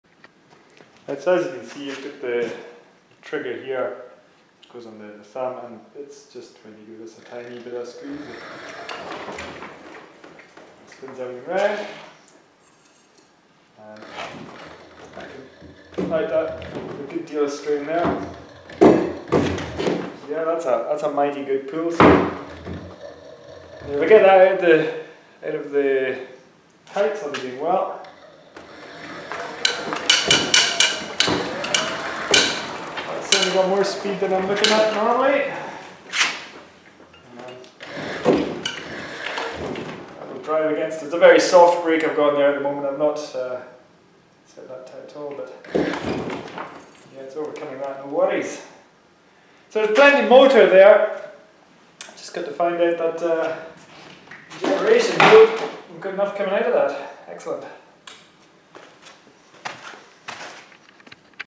First motor test
A tension crown is threaded onto the wheel... it holds the main lift kite line. The torque lines are not driven yet... But a drill attached to a bolt on the bar spins the wheel well... Power measurements will be done in the new year